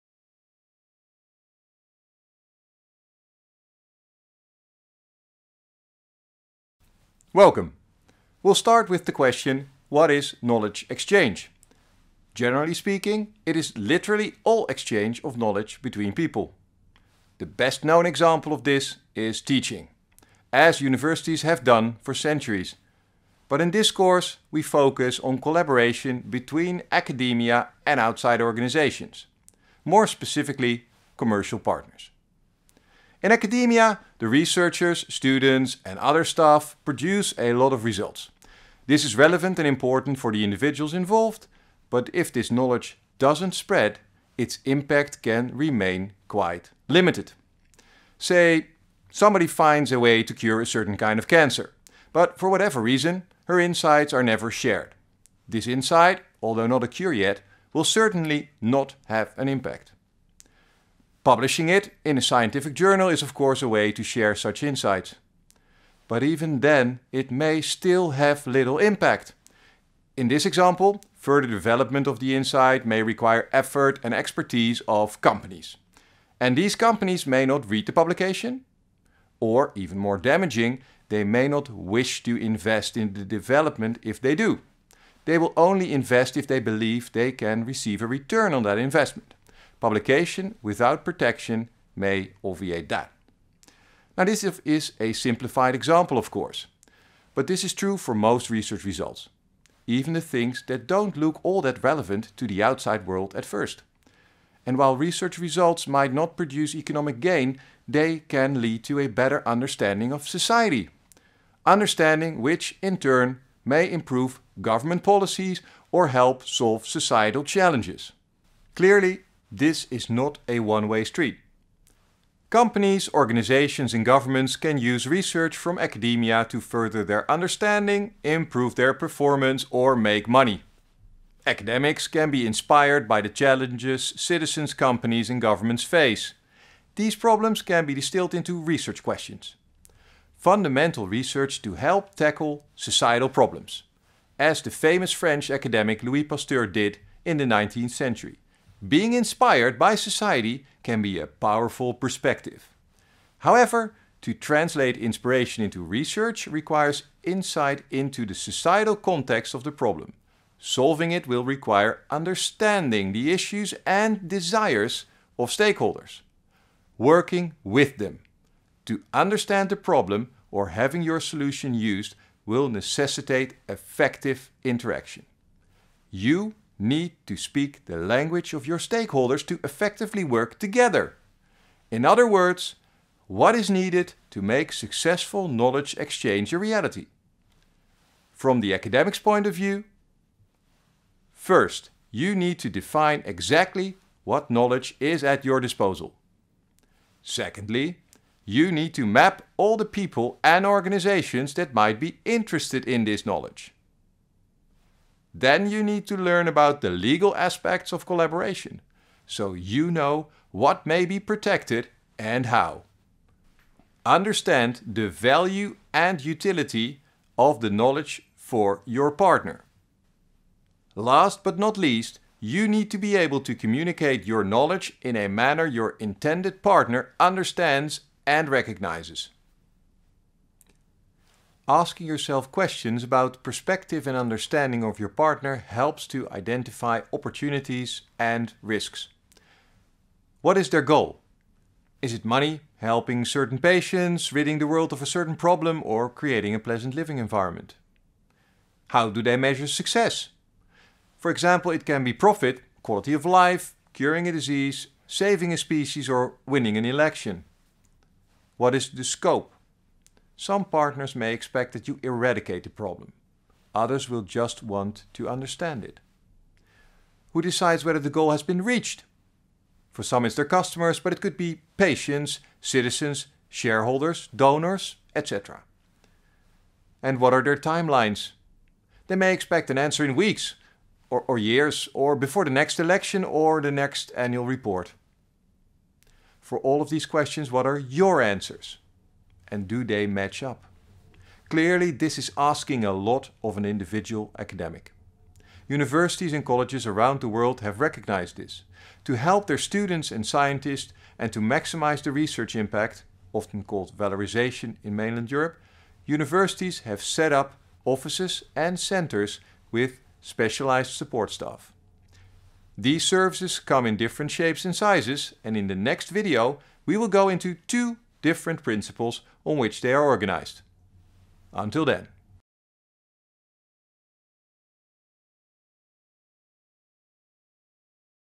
Knowledge Exchange- 1.1 Introduction to knowledge exchange
This lecture is part of a Coursera course called Knowledge exchange: Using, Protecting and Monetizing ideas with Third Parties. It is given by Prof. Dr.  Gert-Jan van Ommen,
and colleagues from Leiden University Medical center and Luris.

Do you want to learn more and practice with the content? You’ll find this online university course for free